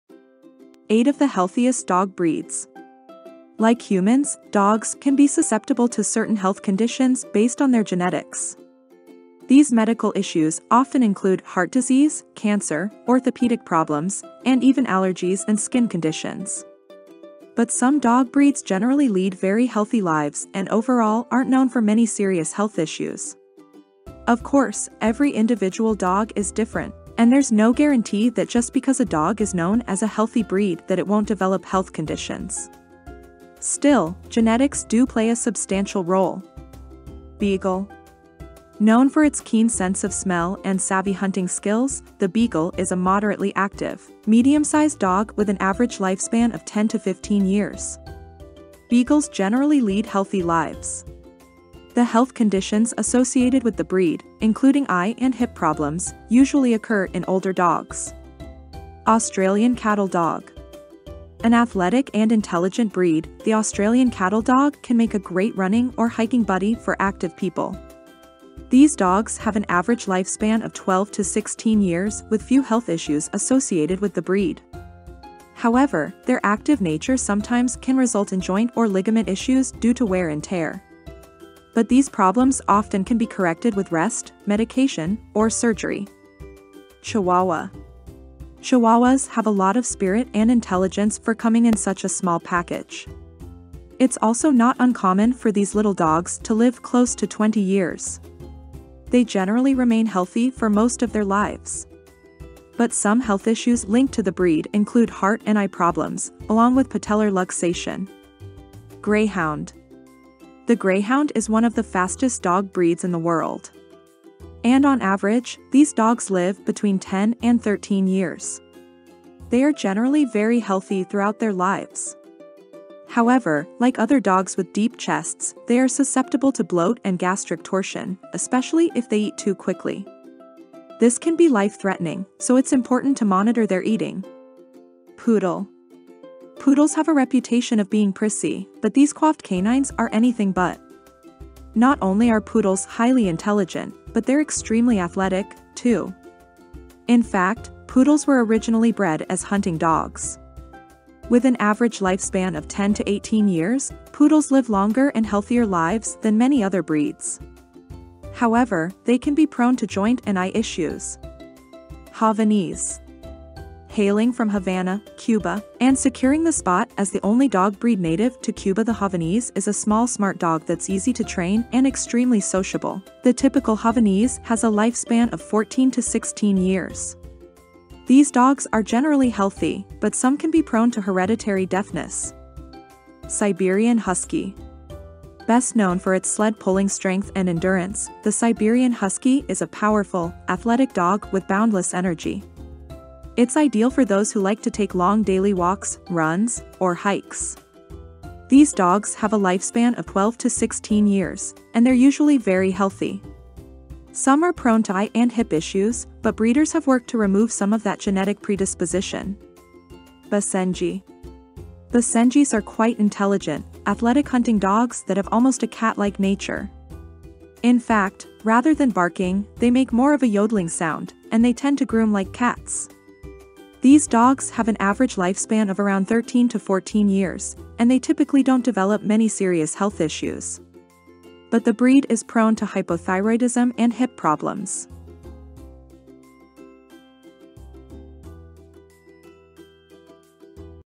8 of the Healthiest Dog Breeds
1. Beagle
2. Australian Cattle
3. Chihuahua
4. Greyhound
5. Poodle
6. Havanese
7. Siberian Husky
8. Basenji